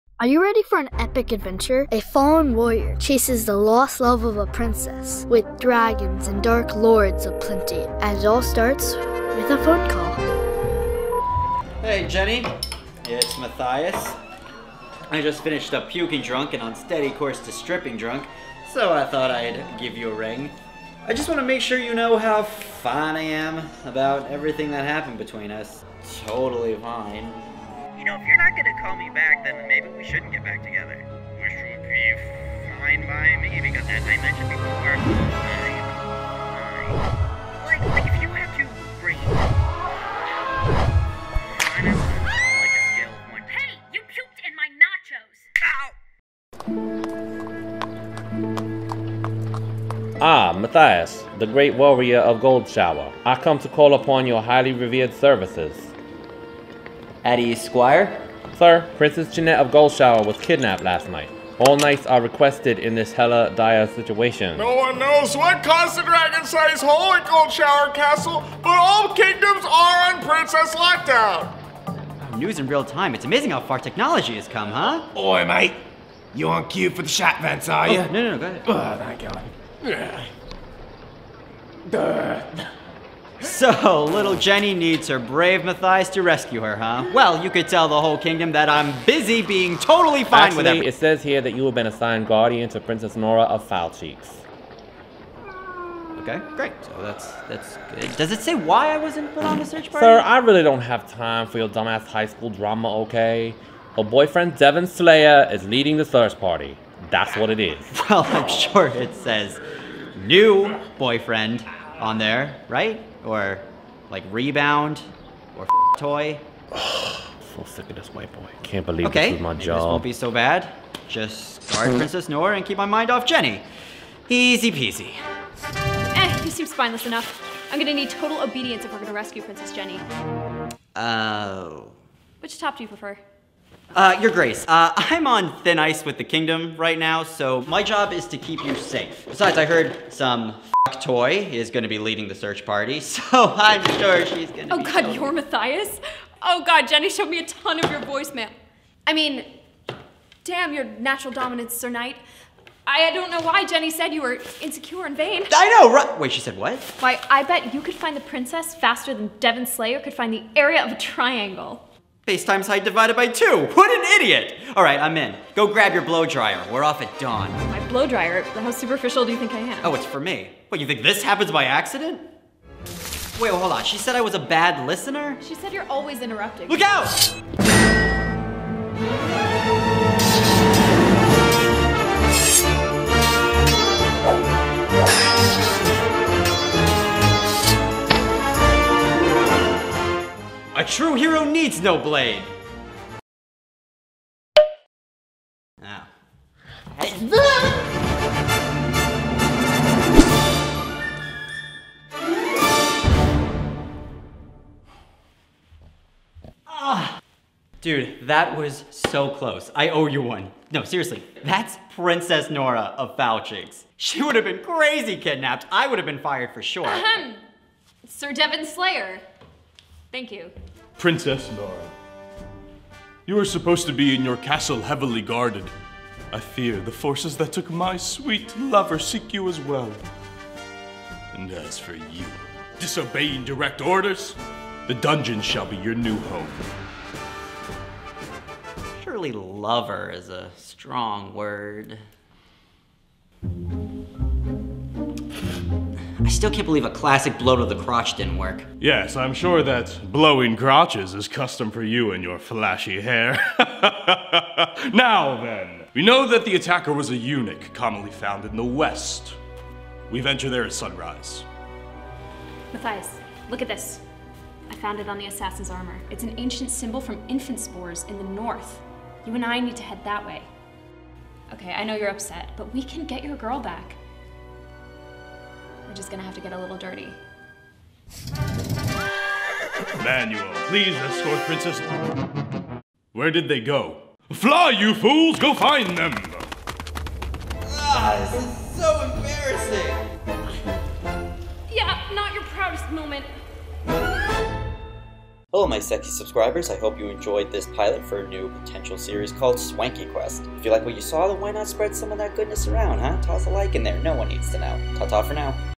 Swanky Quest (Pilot)
Episode 1: Dragons & Cellphones.
This took a lot of work and almost twice as much $$$

See more of me here:
Pokemon Musical - Ash's Dad!

Swanky Films - Matt Giudice
Comedian. Writer.Actor.Editor.Sexy

Crew:
DP - Nick McGee
Dragon Tamer - Eric Ziwak
Also:
Andrew Wood
Michael Braithwaite
Manon
Jenny Rowe
Michael Tedesco

Young Narrator - Keifer Higginbotham
Mathias - Matt Giudice
Jenny - Paige Grimard
Assassin -  Michael Braithwaite
Voicemail Girl - Deanna Giulietti
Squire (voice) - Mark Douglas
News Reporter - Max Knoblauch
Shit Vents User - Dan Johnson
Nora - Nell Kessler
Devin Slayer - Ethan Jovanovic

Special Thanks to:
Key of Awesome
Cara Alpert
Major League of Gaming